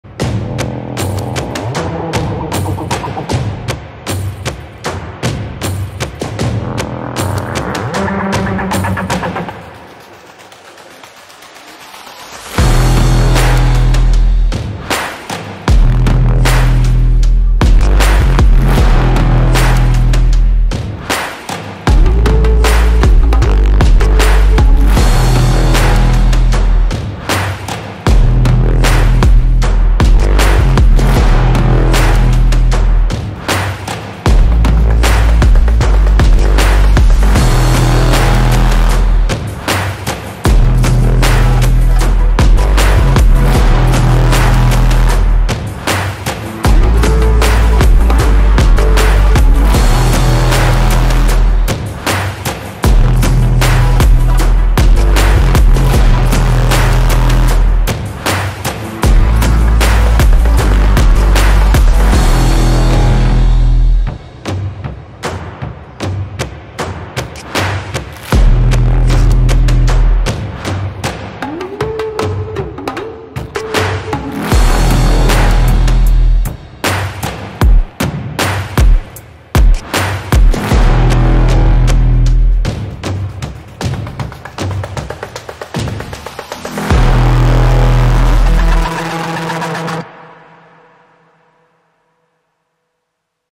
No. 1 Grace completes double-digit-point comeback against Marian
Grace College's men's basketball team completed a second consecutive double-digit-point comeback in a 66-60 victory over RV Marian.